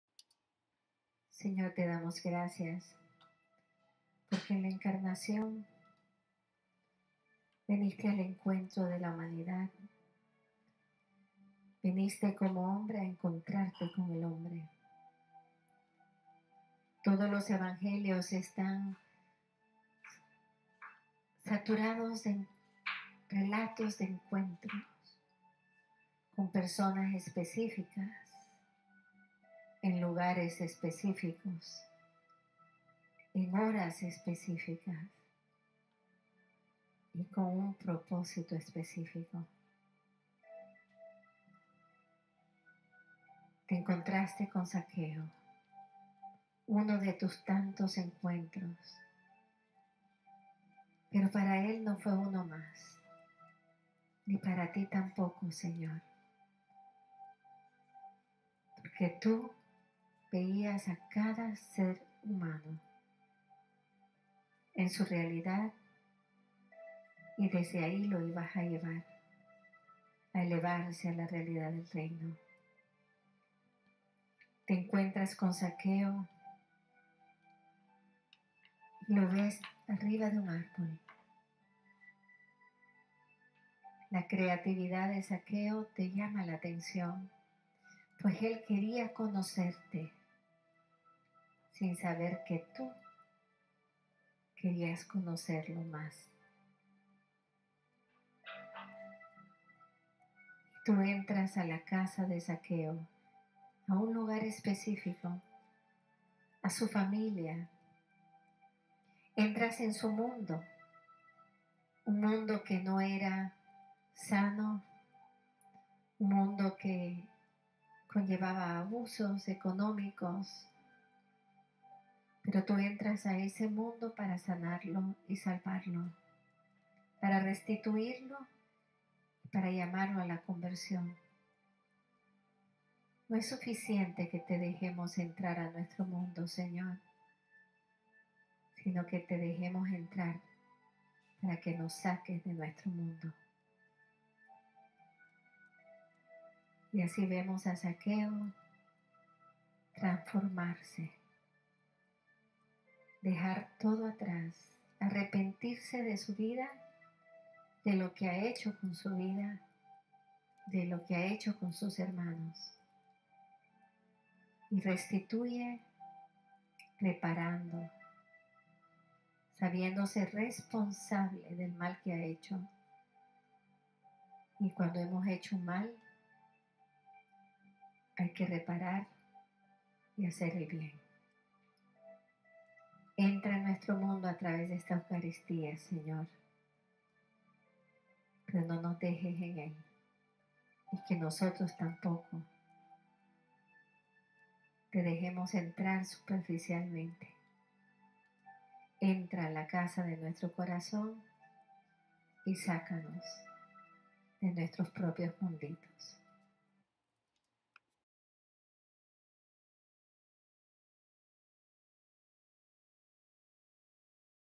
"We are never just one more for Jesus" Spiritual Communion Prayer Mother Adela, SCTJM- Foundress ASL
Spiritual Communion prayer for Oct 30, 2022- 31st Sunday in Ordinary Time

Reading 1, Wisdom 11:22-12:1
Responsorial Psalm, Psalms 145:1-2, 8-9, 10-11, 13, 14
Reading 2, Second Thessalonians 1:11-2:2

Canal de YouTube de las Siervas y Siervos de los Corazones Traspasados de Jesús y María.